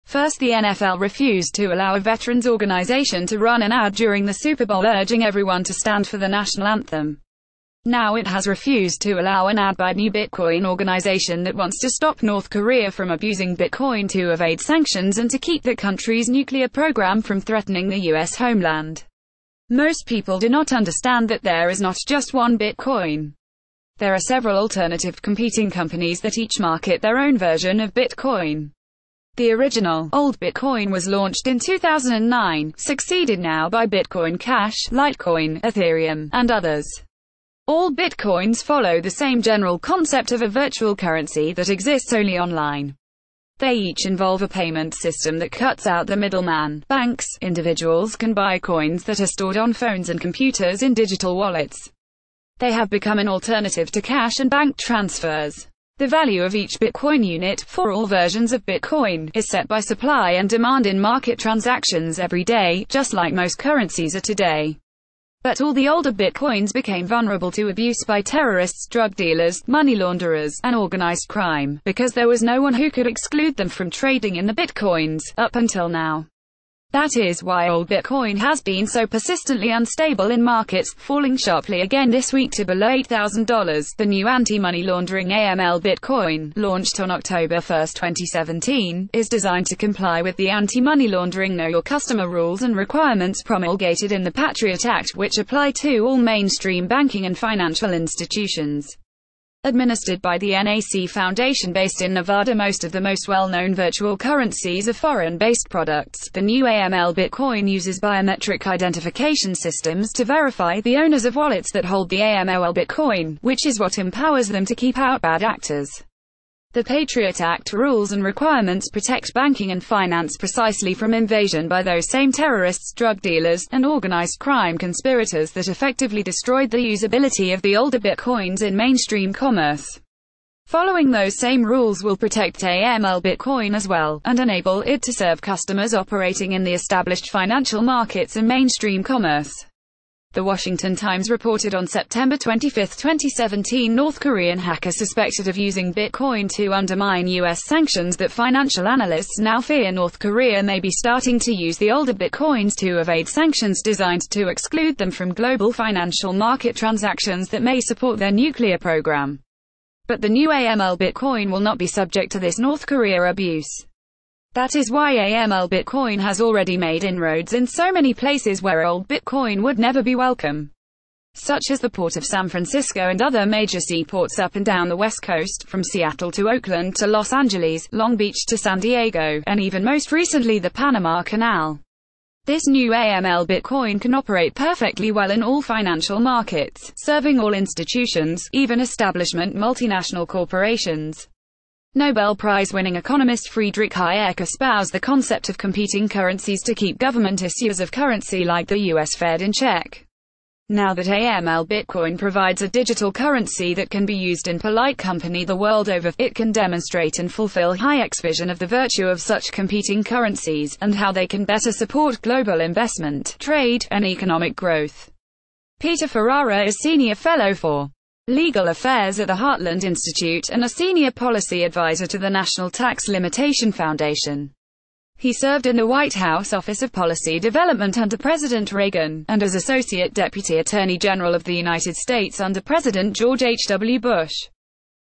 [Breaking News] Bitcoin can shut-down AML new tide old abuse of Bitcoin
First the NFL refused to allow a veterans organization to run an ad during the Super Bowl urging everyone to stand for the national anthem. Now it has refused to allow an ad by a new Bitcoin organization that wants to stop North Korea from abusing Bitcoin to evade sanctions and to keep that country’s nuclear program from threatening the U.S. homeland. Most people do not understand that there is not just one Bitcoin. There are several alternative, competing companies that each market their own version of Bitcoin. The original, old Bitcoin was launched in 2009, succeeded now by Bitcoin Cash, Litecoin, Etherium, and others. All Bitcoins follow the same general concept of a virtual currency that exists only online. They each involve a payment system that cuts out the middle man — banks.  Individuals can buy coins that are stored on phones and computers in digital wallets. They have become an alternative to cash and bank transfers. The value of each Bitcoin unit, for all versions of Bitcoin, is set by supply and demand in market transactions every day, just like most currencies are today. But all the older Bitcoins became vulnerable to abuse by terrorists, drug dealers, money launderers, and organized crime, because there was no one who could exclude them from trading in the Bitcoins, up until now. That is why old Bitcoin has been so persistently unstable in markets, falling sharply again this week to below $8,000.The new Anti-Money Laundering (AML) BitCoin, launched on October 1, 2017, is designed to comply with the anti-money laundering know you...